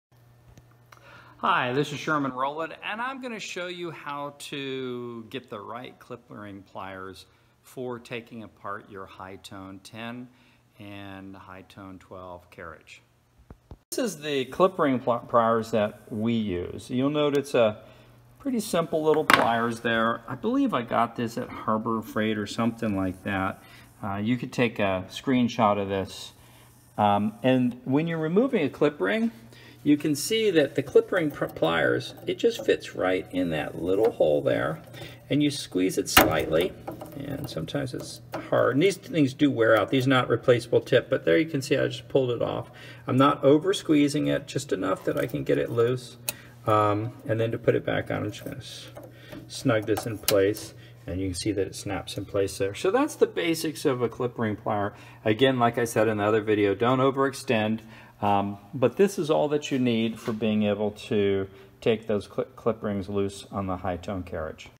How to select a clip ring pliers for removing your HighTone Slab Saw crossfeed rails
This video gives you a brief overview of how to select and use a clip ring pliers for removing the clip rings holding the crossfeed rails from your Highland Park HighTone 12 and 14 slab saw